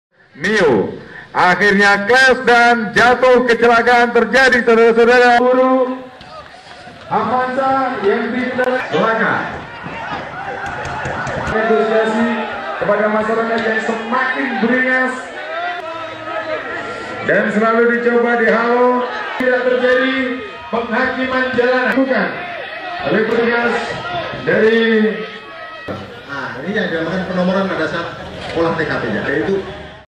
Polisi Gelar Simulasi Penanganan Laka Lantas
Metrotvnews.com, Tulungagung: Rangkaian kejadian kecelakaan yang melibatkan sepeda motor dan mobil ini hanya merupakan simulasi saat launching progam Motor Traffic Accident Service yang digelar Polres Tulungagung Kamis (24/4/2014). Program kecepatan datang di tempat kejadian perkara ini dilaksanakan karena dari evaluasi di lapangan terutama kejadian laka lantas bila polisi tidak segera datang di lokasi kejadian sering terjadi sikap anarkisme masa yang berlebihan.